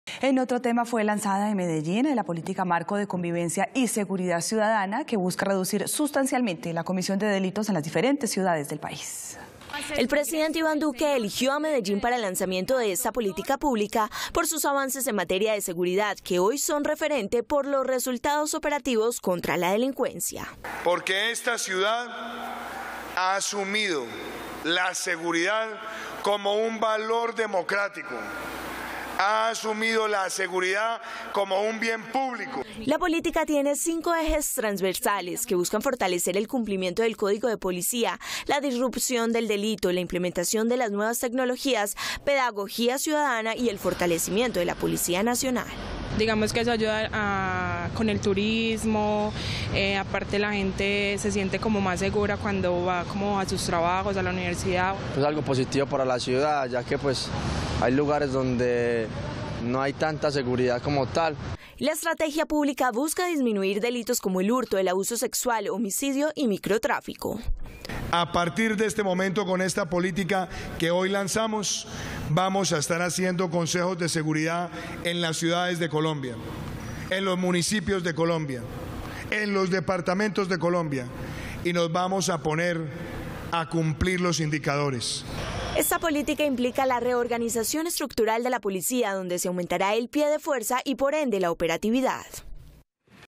Alcances de la nueva Política de Convivencia de Seguridad ciudadana [Noticias] - Telemedellín
Fue lanzada en Medellín la política marco de convivencia y seguridad ciudadana que busca reducir sustancialmente la comisión de delitos en las diferentes ciudades del país. Esta estrategia contempla algunas reformas en la operatividad nacional.